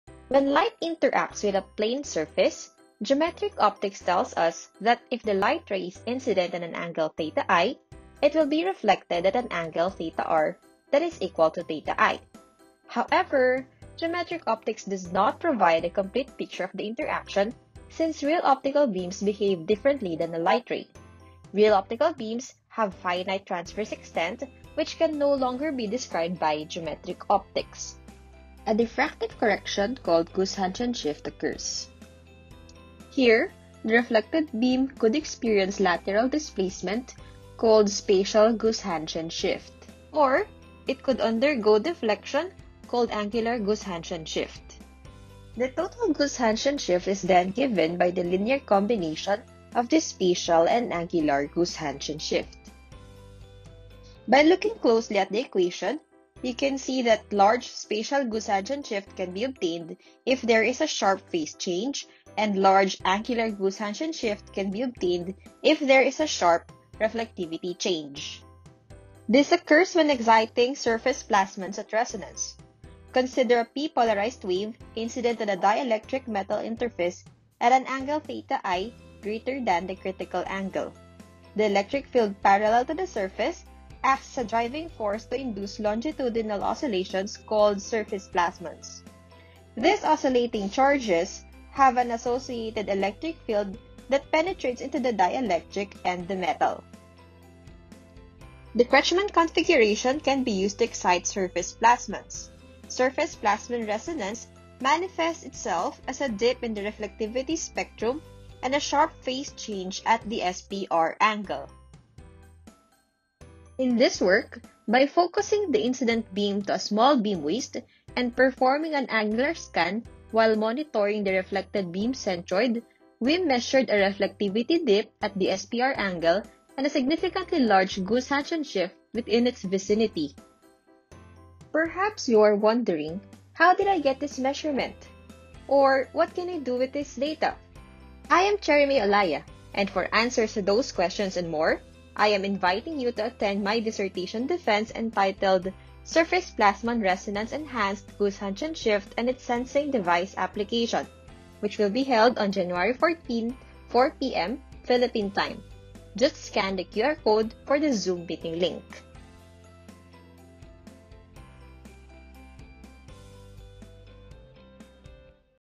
Surface Plasmon Resonance Enhanced Goos-Hanchen Shift and its Sensing Device Application [CM Olaya]
14 January 2022 (Friday)
PhD Dissertation Defense
4:00 -- 6:00 pm

Olaya, Cherrie May

Title: Surface Plasmon Resonance Enhanced Goos-Hanchen Shift and its Sensing Device Application

Adviser: Hermosa, Nathaniel II
Co-Adviser: Tanaka, Takuo
Reader: Vega, Michael Francis Ian II
Examiners: Balela, Donabelle (DMMME), Estacio, Elmer